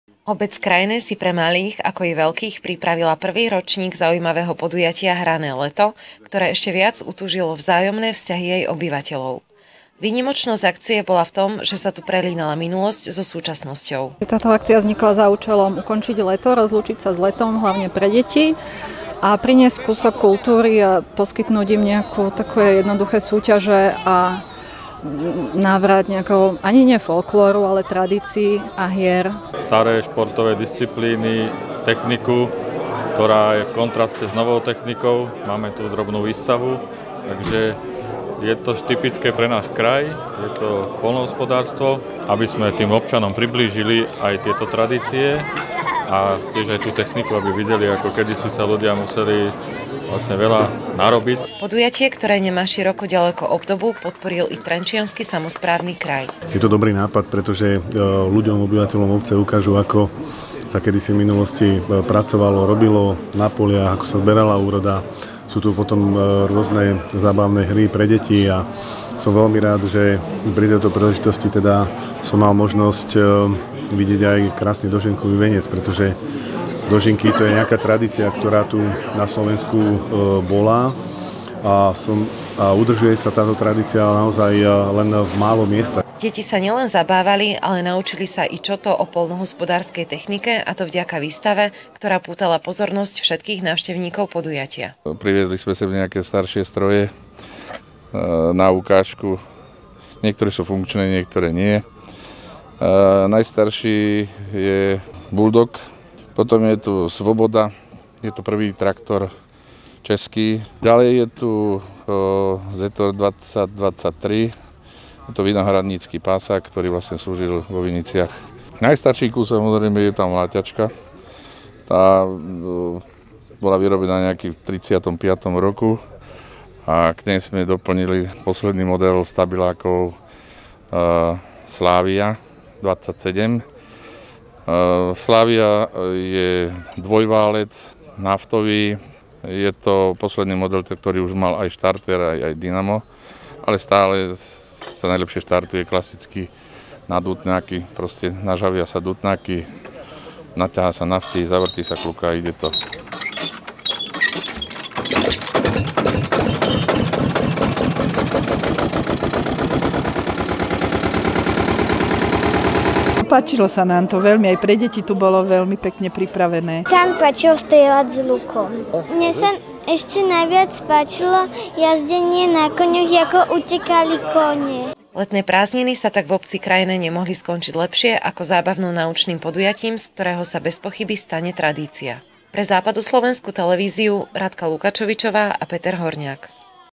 Hravé leto v Krajnom
Výstava poľnohospodárskej techniky, zábavné hry pre deti, či letné kino. To všetko čakalo na účastníkov jedinečného podujatia Hrané leto v Krajnom, ktoré sa tento rok konalo po prvýkrát a hneď si získalo svojich priaznivcov.